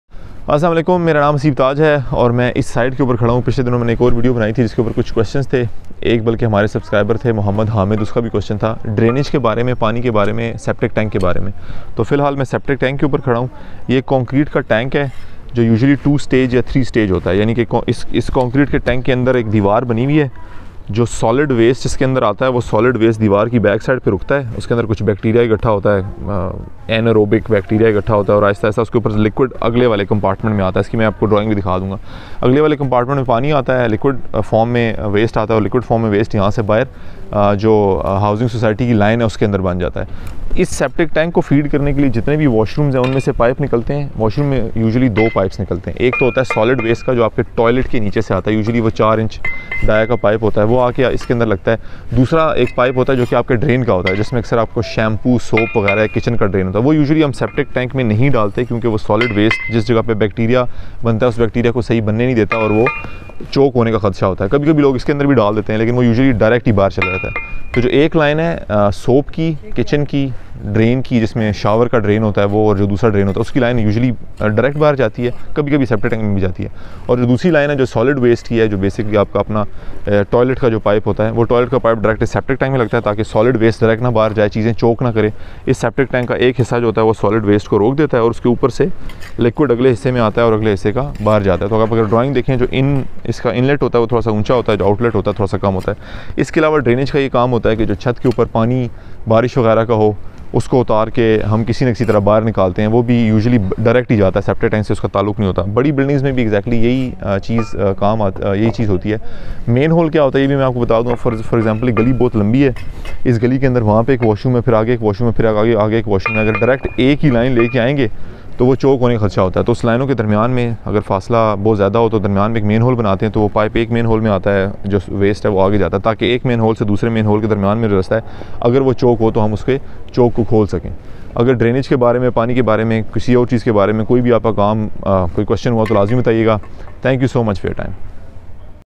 5, 10 Marla House Construction Procedure (Part 2) Septic Tank and Drainage
Overall drainage and the construction of septic tank is an important part of the construction process and needs to be handled properly. The septic tank is responsible for transforming solid waste to liquid waste so that it can be handled properly in the sewerage lines.